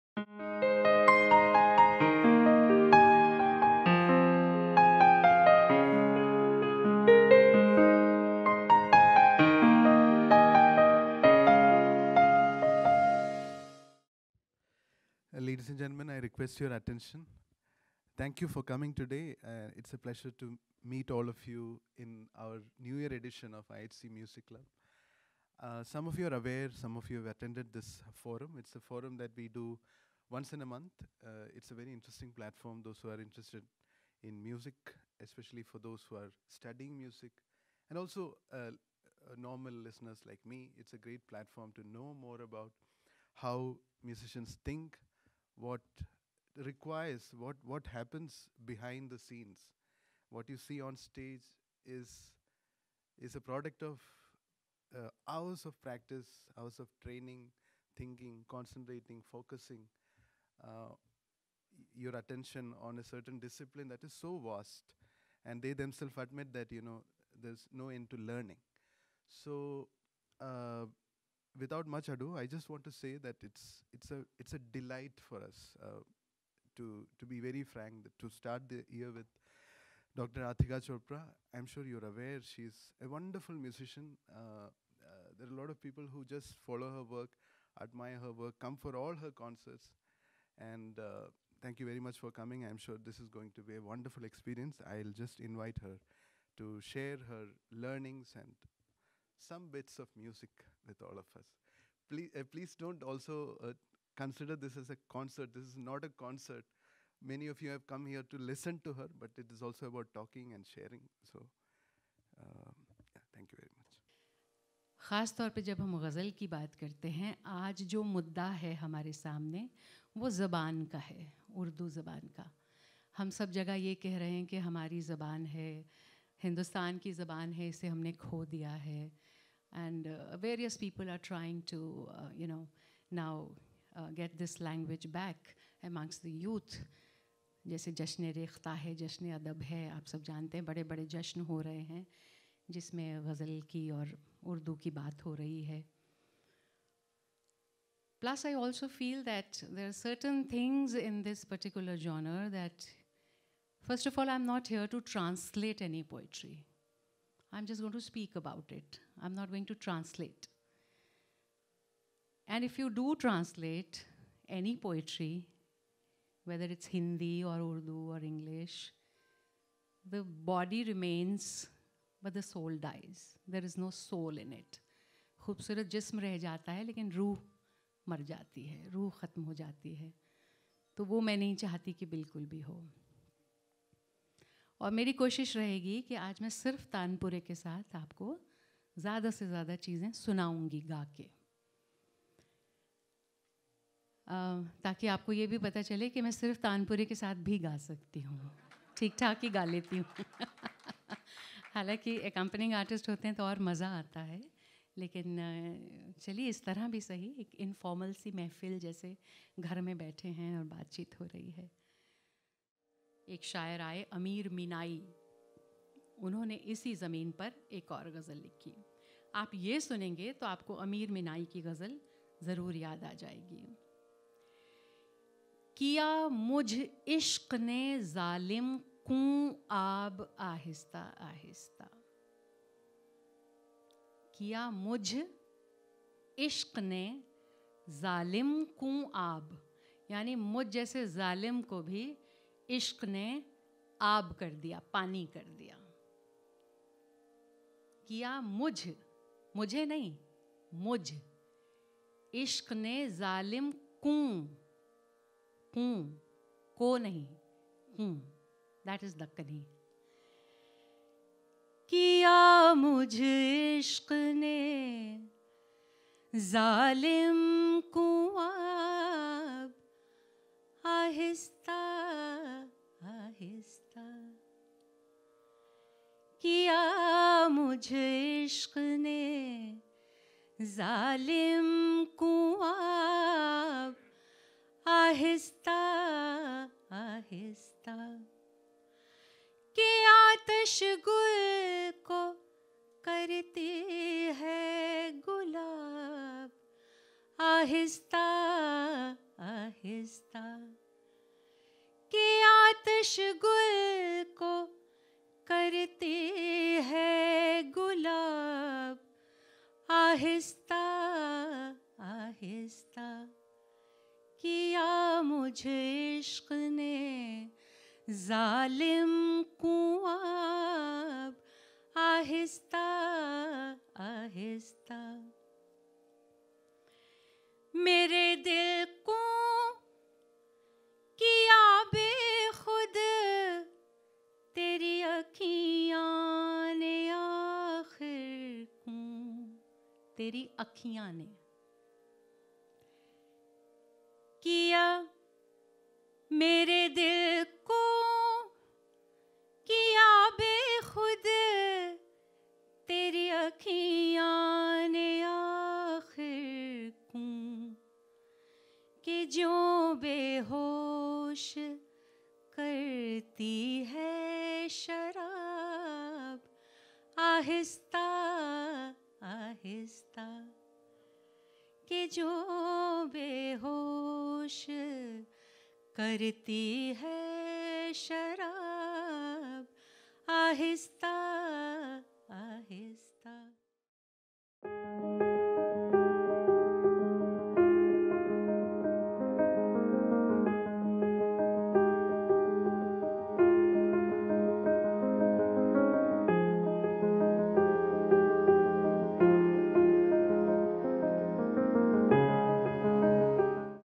11 January 2019 - MUSIC & TALK - IHC MUSIC CLUB: Listen, Share and Discuss
MUSIC & TALK - IHC MUSIC CLUB: Listen, Share and Discuss. A lecture and demonstration by Ghazal vocalist Dr. Radhika Chopra on Ghazal as a genre